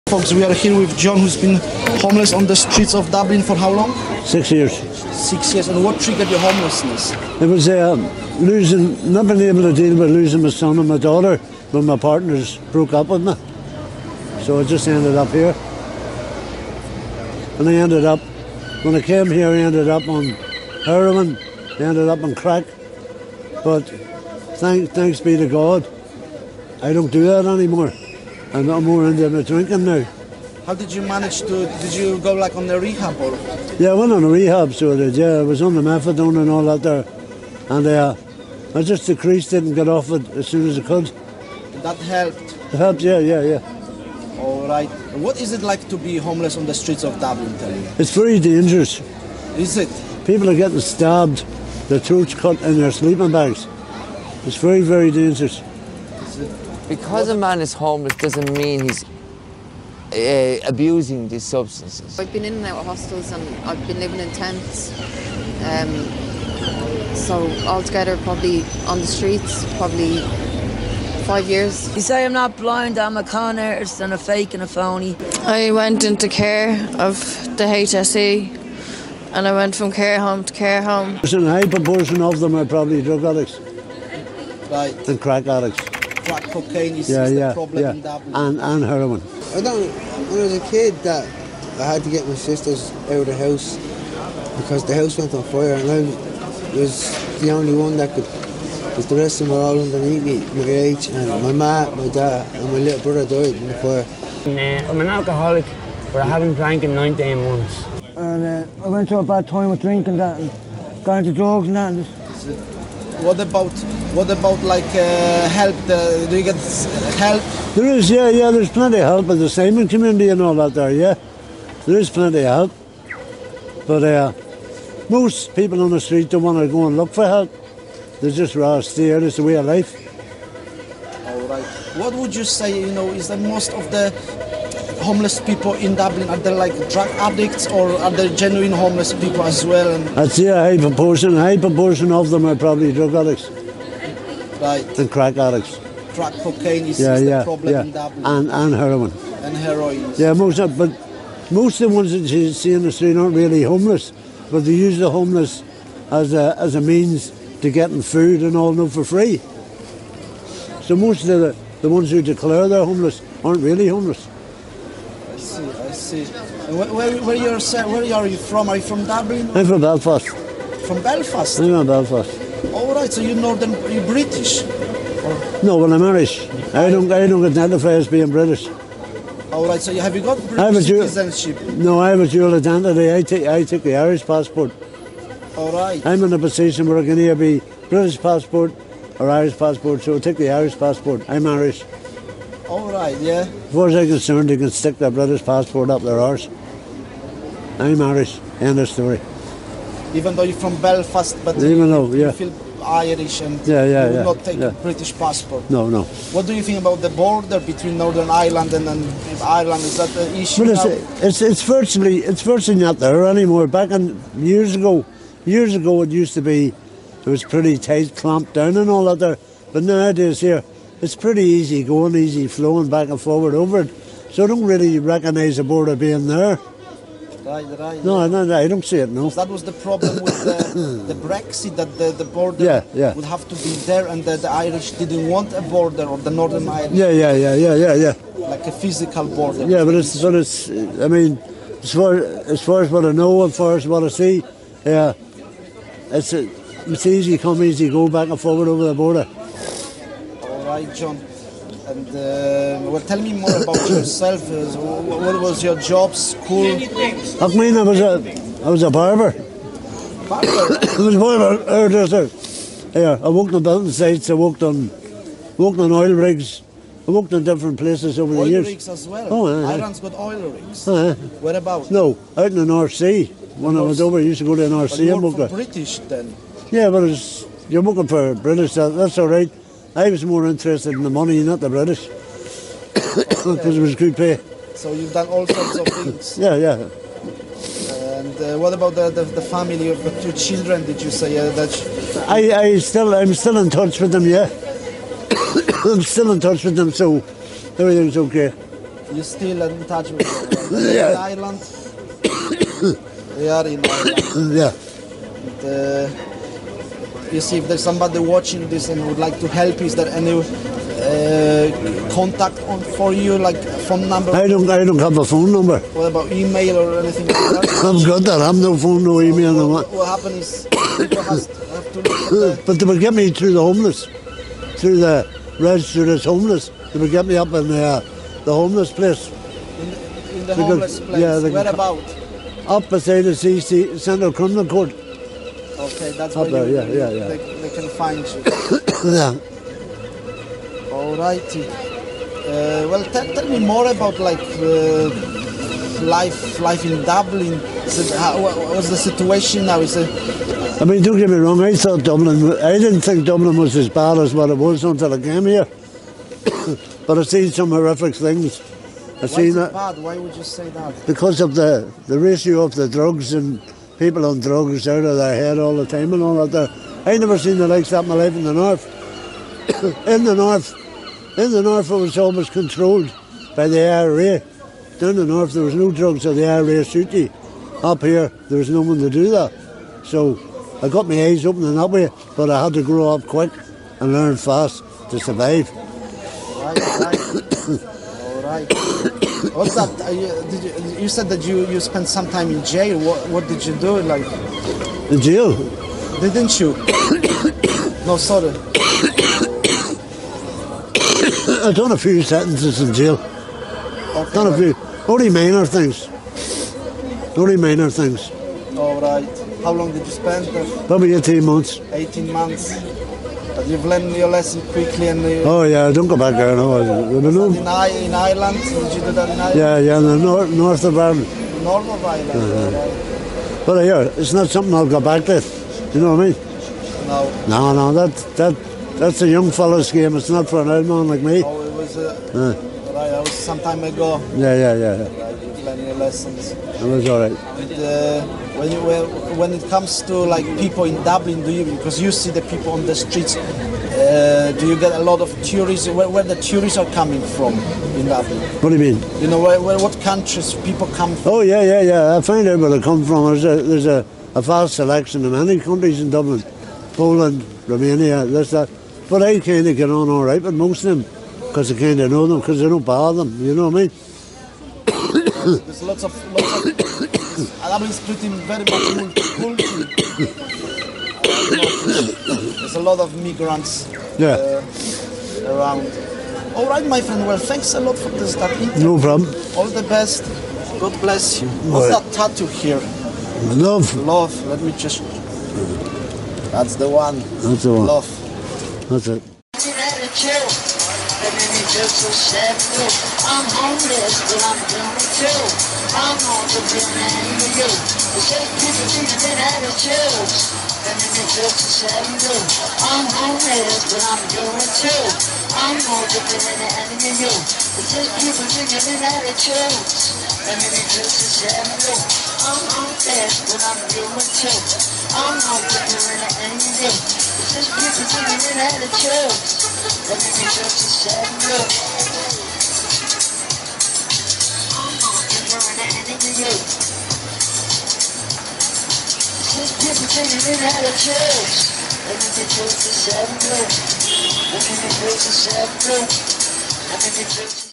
Meet John - Born in Belfast, now on the streets - Dublin, Ireland
Zupa's walks

Music

Silesian Schalger